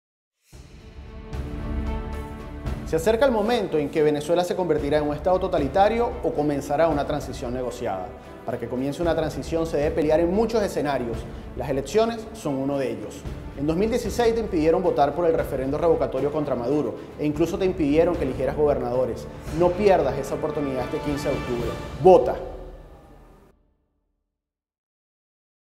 ¿Por qué votar? - Eugenio Martínez
"No pierdas esa oportunidad el 15Octubre, vota" dijo Eugenio Martínez, invitando a todos los venezolanos a ejercer su derecho este domingo en las EleccionesDeGobernadores.